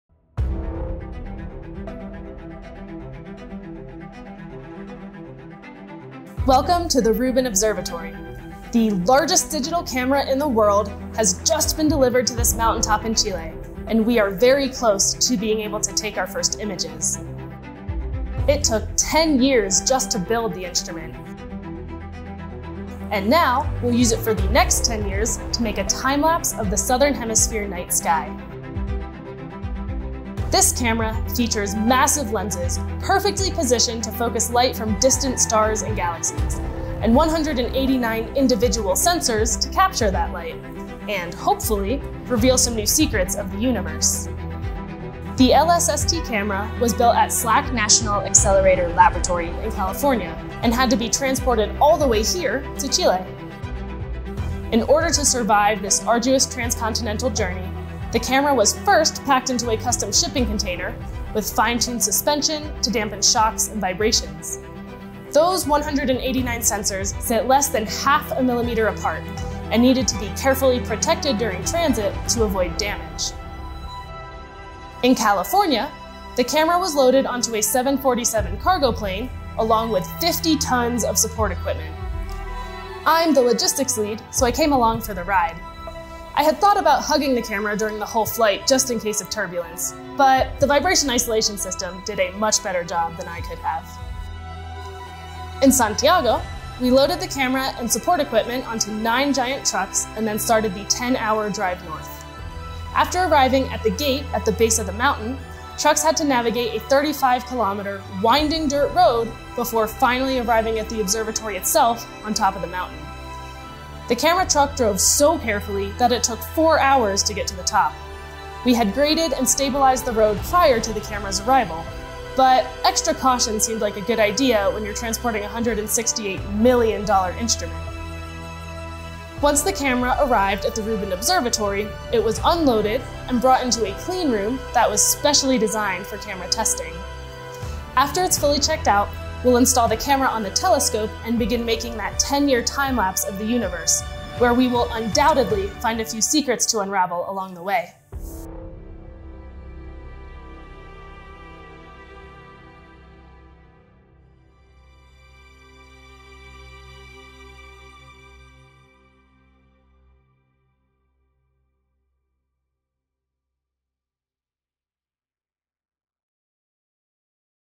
How did SLAC ship the largest digital camera to Chile?
Margaux Lopez is the logistics lead for shipping the LSST Camera to Chile. The world's largest digital camera, crafted at SLAC National Accelerator Laboratory in California, has arrived at Rubin Observatory in Chile after an intricate journey along the west coast of the Americas. It was securely encased in a custom shipping container with advanced suspension to mitigate shocks and vibrations, essential for protecting its 189 closely spaced sensors. The camera was flown to Santiago on a 747, accompanied by 50 tons of support gear. From there, it was trucked on a 10-hour drive and then up a 20-mile winding mountain road, taking four hours due to cautious driving. The $168 million instrument's arrival marks a critical step before it begins capturing a decade-long timelapse of the Southern night sky.

Video is produced by SLAC National Accelerator Laboratory where the LSST camera was built.

Written, edited and produced by Olivier Bonin | SLAC
Multi-year timelapse of the camera construction by Tom Glanzman | SLAC
San Francisco Airport videos by Jim Gennsheimer
LSST camera group photo by Jacqueline Orrell | SLAC
LSST camera packing videos by Sam Soon | SLAC